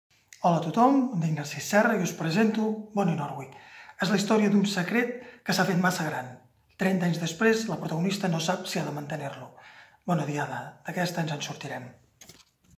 Serra, Narcís
Narcís Serra presenta el seu llibre "Bonnie Norwick" a la Revetlla virtual de Sant Jordi 2020 de la Biblioteca de Figueres.